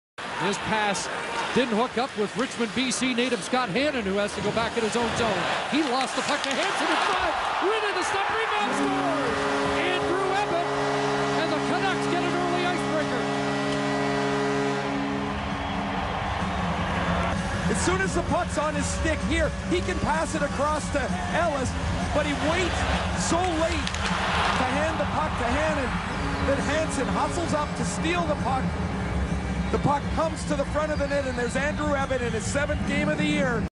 Andrew Ebbett - All Goals - 2013 Season
Andrew Ebbett's lone goal from the 2013 season.

Goal:
#1 Canucks VS Predators 3/14/13

Footage from TSN

Property of the NHL